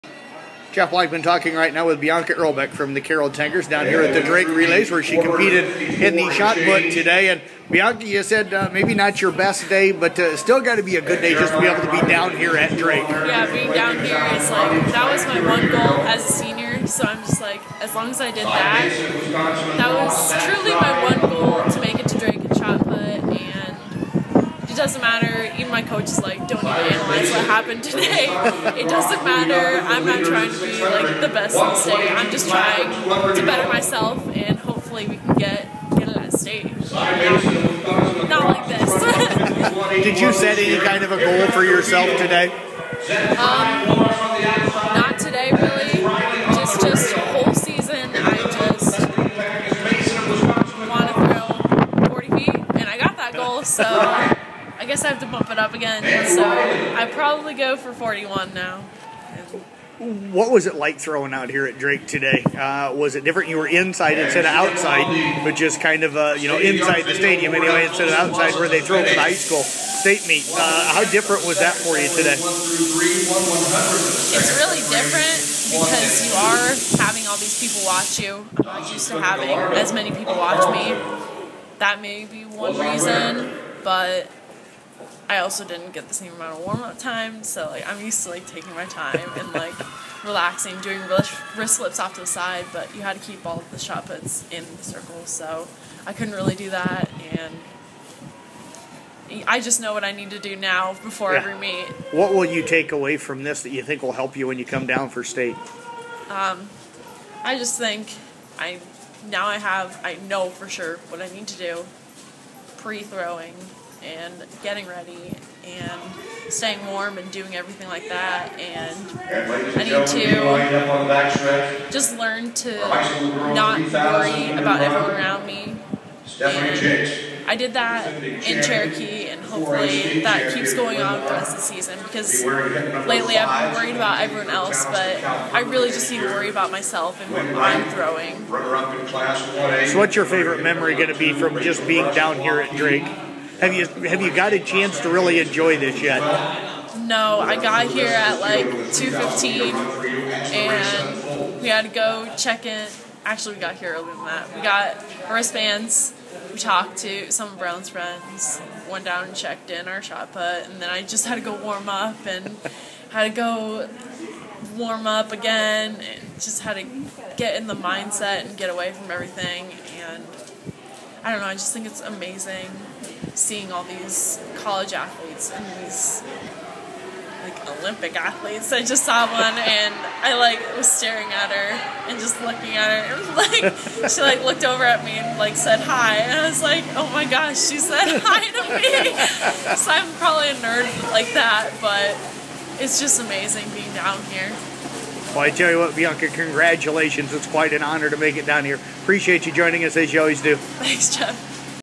Interview with Bianca Irbeck with the Carroll Tigers
Jeff Blankman talks with Bianca Irlbeck at Drake Relays. Irlbeck competed in Shot Put